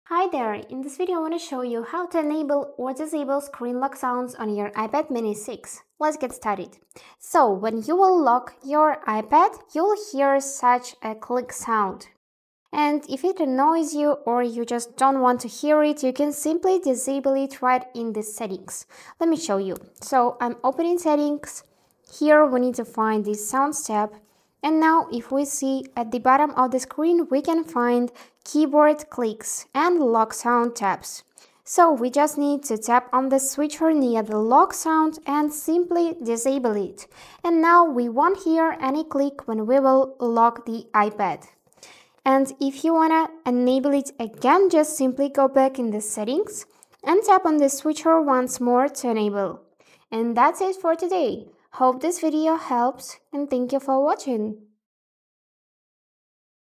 How to Enable/Disable Screen Lock Sounds on iPad Mini 6
Are you looking to customize your iPad Mini 6 experience by adjusting the screen lock sounds? In this video, we'll guide you through the simple steps to enable or disable the screen lock sounds on your device. Learn how to make your iPad more personalized by choosing the right sound settings that fit your style. Whether you prefer a silent mode for your iPad Mini 6 or enjoy the satisfying click of the lock sound, this tutorial has you covered! Follow along and discover how to enhance your iPad Mini 6 experience today.